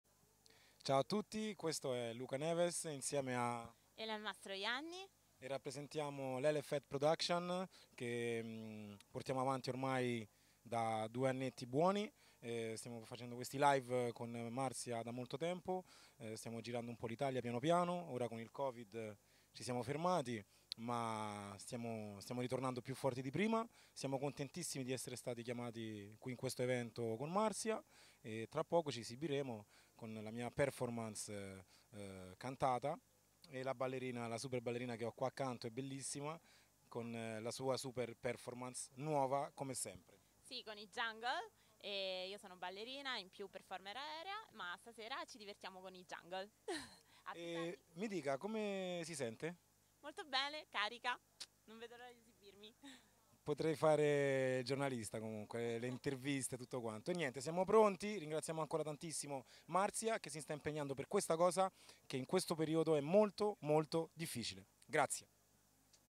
NewTuscia Tv: Intervista a rapper Luca Neves e ballerina
Finale regionale Lazio di Un Volto x Fotomodella